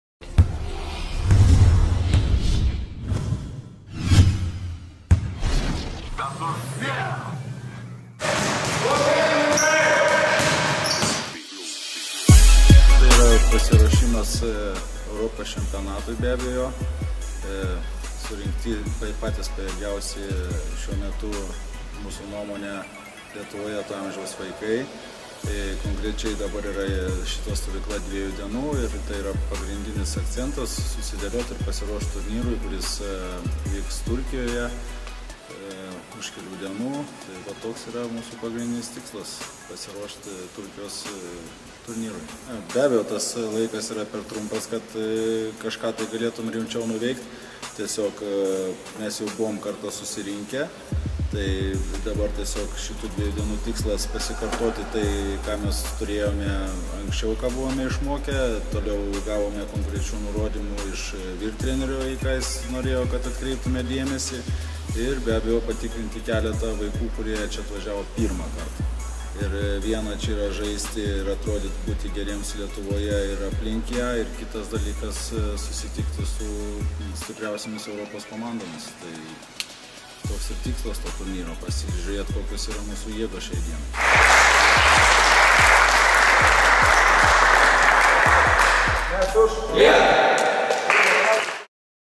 U16 vaikinų krepšinio rinktinė išbandys jėgas Turkijoje
Šių metų šešiolikmečiai (U16), Europos čempionatui besirengiantys vaikinai, išvyksta į prestižinį tarptautinį turnyrą Samsuno mieste, Turkijoje.